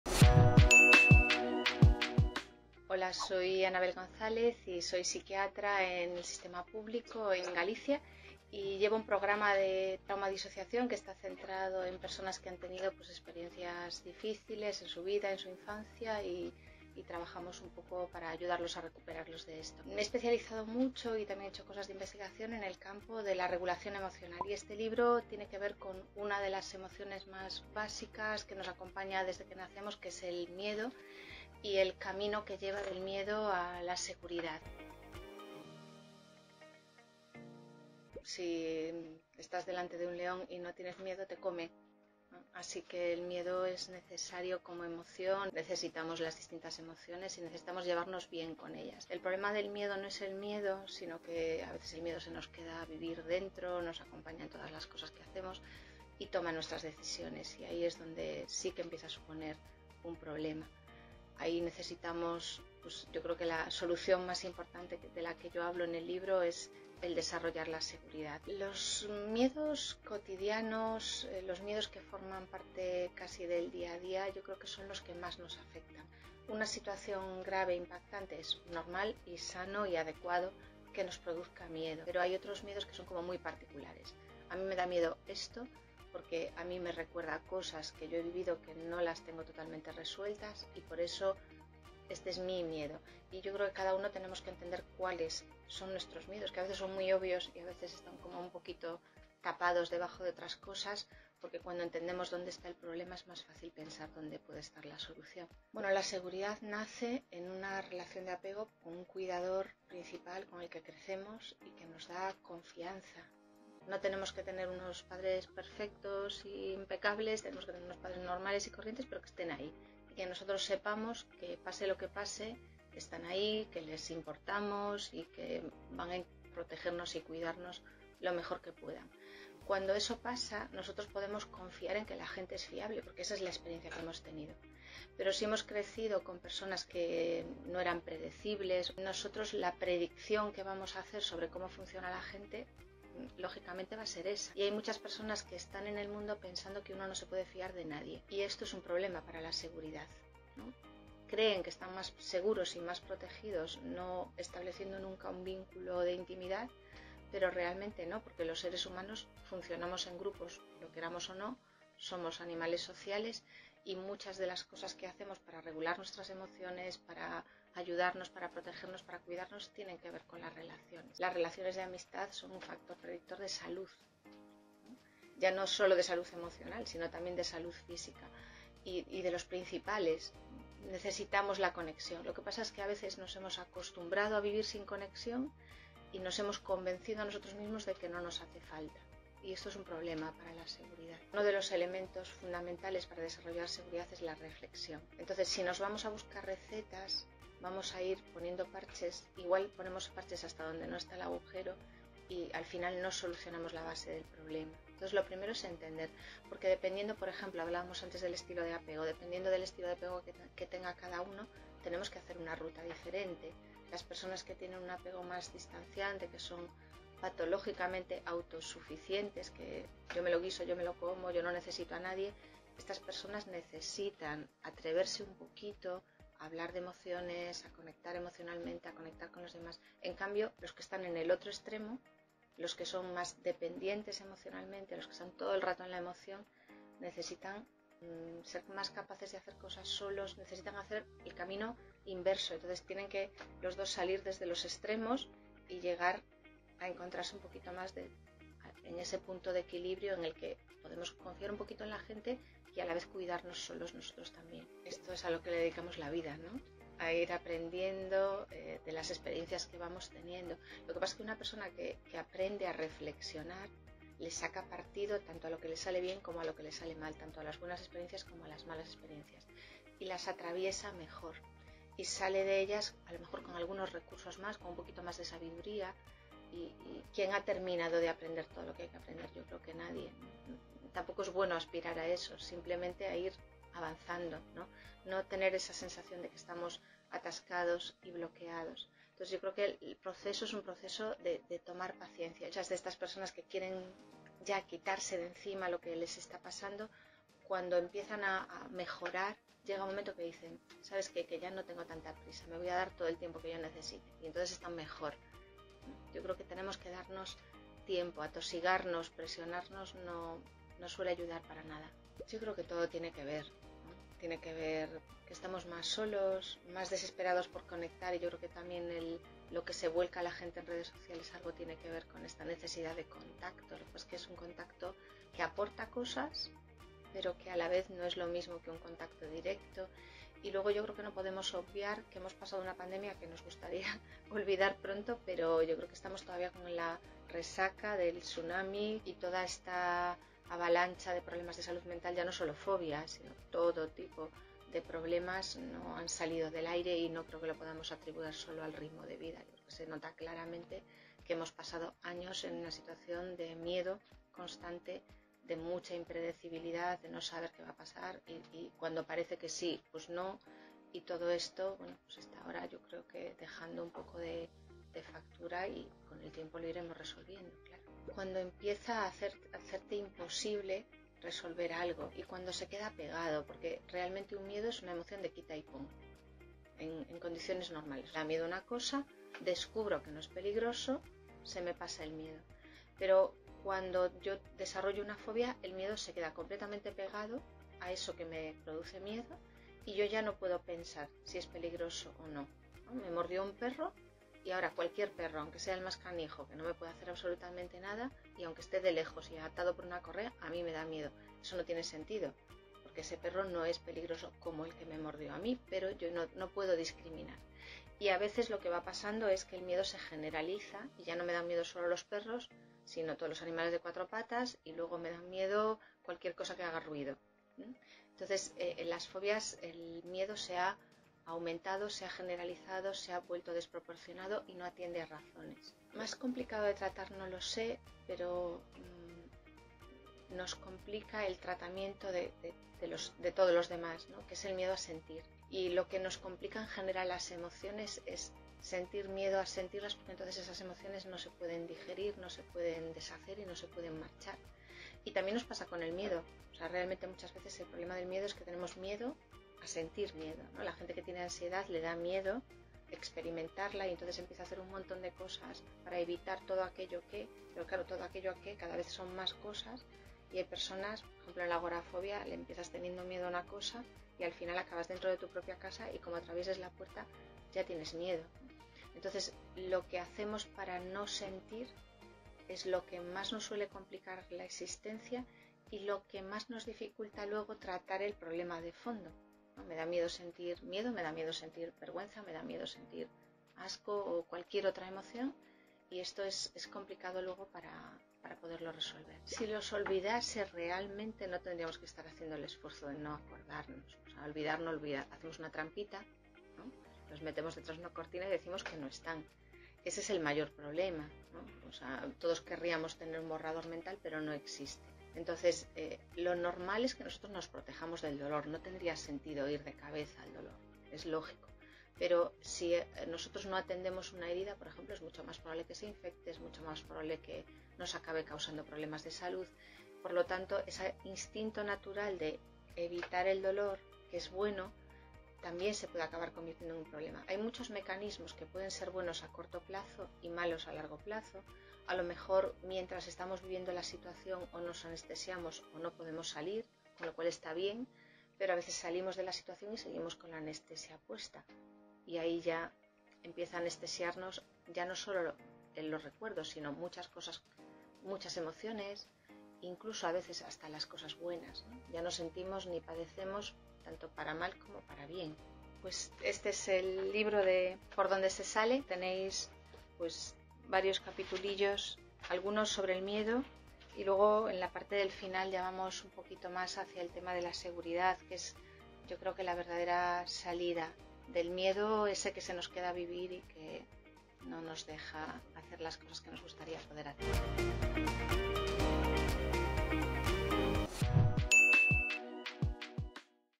Entrevista a la psiquiatra Anabel González
Entrevistamos a la psiquiatra Anabel González para que nos explique cómo deshacer el miedo, aliviar el malestar psicológico y adquirir un apego seguro.